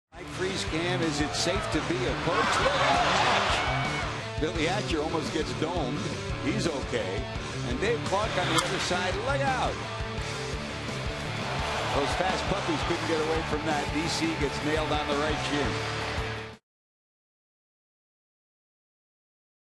2010/09/18 Is it safe to be a coach?
CIN@HOU: Coaches come close to getting hit